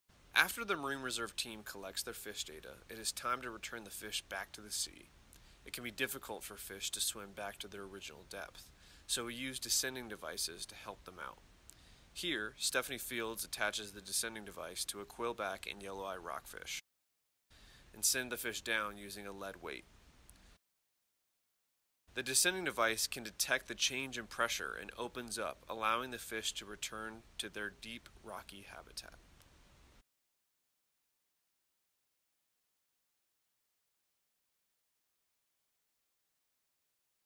ODFW Marine Reserves Program: Recompressing Rockfish
Learn about rockfish recompression as the ODFW Marine Reserves Team demonstrates how we use 'descending devices' to help assist fish swim back down to depth. Fish that have swim bladders - including most Rockfish species - often experience barotrauma when brought to the surface. These are physiological impairments due to expanding air. As a result, we use the Seaqualizer descending device to clip onto a fish's lip and push them back down to depth. Seaqualizers automatically release at preset depth limits by responding to increased water pressure. We routinely use these devices on our hook and line surveys to ensure high survival rates of all encountered fishes.

Studies have shown that rockfish have high survival rates if quickly released back to depth. With your help, fishing even more sustainable in Oregon if prohibited species are released using a descending device.